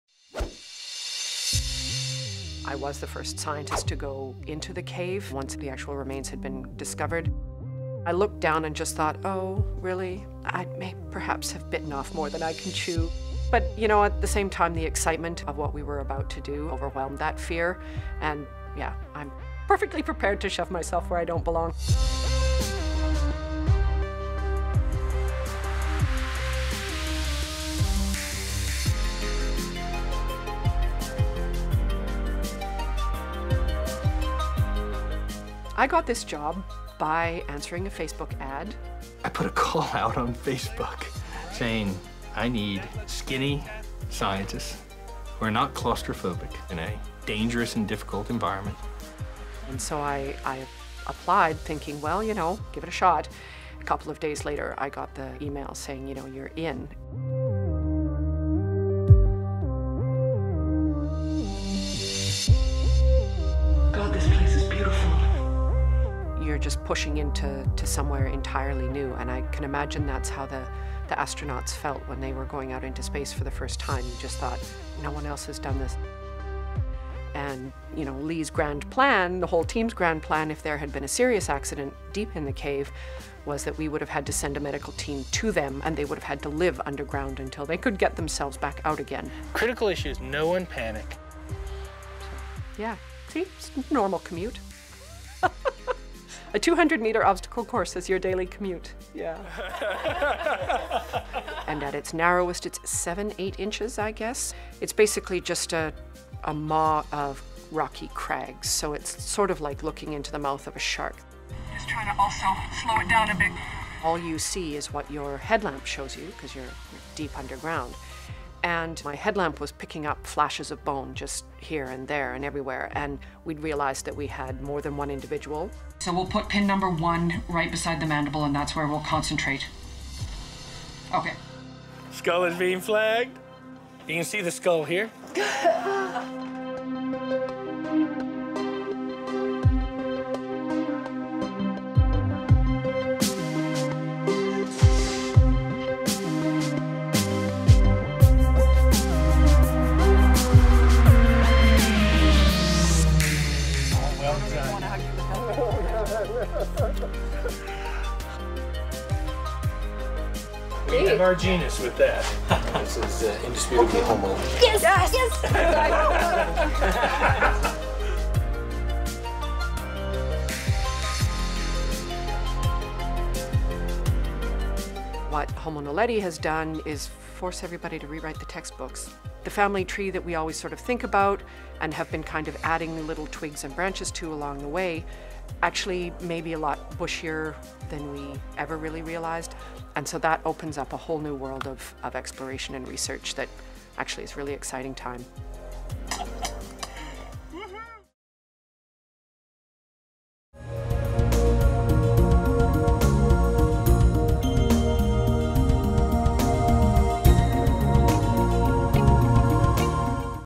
Squeezing Through Rocky Caves to Find Ancient Skeletons | Expedition Raw
Watch the real-life "bone collector" in action. Biological anthropologist and National Geographic Emerging Explorer Marina Elliott ventured a hundred feet deep into an ancient cave, at times squeezing through passages only eight inches wide, to recover the remains of what turned out to be a newly discovered, extinct human ancestor.

About Expedition Raw:
Surprises, challenges, and amazing behind-the-scenes moments captured by National Geographic explorers in the field.

Working with expedition leader and National Geographic Explorer-in-Residence Lee Berger, Elliott led an excavation team into Rising Star Cave, where cavers had previously come across unidentified bones. Elliott expected a relatively quick job of retrieving fossils belonging to a single individual but "when I first started looking into that space, my headlamp was picking up flashes of bone just here and there and everywhere," she recalls. "We recovered around 1,500 fragments from that first expedition, in an area smaller than a child's sandbox." Upon examination, the scientists determined that the fossils belong to the human genus—but not to a species already known to science.

The discovery of our new but extinct family member Homo naledi—named after Rising Star Cave, as naledi means "star" in a local South African language—is turning our understanding of our past on its head just a bit. Homo naledi appears to be one of the most primitive known species in the human genus, with a small brain and ape-like features. But H. naledi also has some more humanlike features that distinguish it from any other known early human ancestors. It has curved fingers good for climbing, feet and legs suited for long-distance walking, and H. naledi may have even engaged in ritualized behavior.

"There's a lot of books about human origins and human evolution. For a long time, I think we thought we sort of wrapped it up. What Homo naledi has done is kind of forced a whole scale rethink of that. The family tree that we always think about, and have been adding little twigs and branches to along the way, actually may be a lot bushier than we ever really realized. In fact, that whole tree analogy may not be a good analogy at all. A lot of these branches actually rejoined each other and became something else. The relationships of past species and past populations is a lot more complicated than we had originally assumed.

"We have 15 individuals, ranging in age from neonates to older adults. The whole assemblage is something that we didn't think we'd ever get in paleoanthropology. We're used to dealing with very small scraps of material. What this is doing is a real wake up call to the discipline," says Elliott.

But how did so many of our ancestors end up in the underground labyrinth? "Really, there's no other explanation than they were deliberately deposited there. Whether they were dragged all the way in or whether they were sort of tumbled in is another question," Elliott says, musing on the possibility of ritualized behavior. It's actually a lack of evidence that leads scientists to believe our ancestors were purposely placed in Rising Star. "We have no buck, or deer, no ungulates in there, not a baboon bone, not a porcupine, none of these species that often frequent caves in the first place, are in there. We have no signs of carnivore activity on the bones. Even if there had been an opening from above, such as a sink hole, there are very few sink holes that selectively kill only one species." Elliott explains.

While Homo naledi may be the discovery of a lifetime—or perhaps, many lifetimes—Rising Star may hold yet more clues to our ancient past. "The space that we excavated is only maybe a tenth of the surface of that chamber floor," says Elliott.  "There's a lot more material to recover. It's going to be decades of work." Elliott is leading those ongoing excavations.

Marina Elliott is a grantee of National Geographic's Expeditions Council.

To learn more about the science and exploration supported by the nonprofit National Geographic Society,

VIDEO PRODUCER/EDITOR: Nora Rappaport
SERIES PRODUCER: Chris Mattle
ASSOCIATE PRODUCER: Elaina Kimes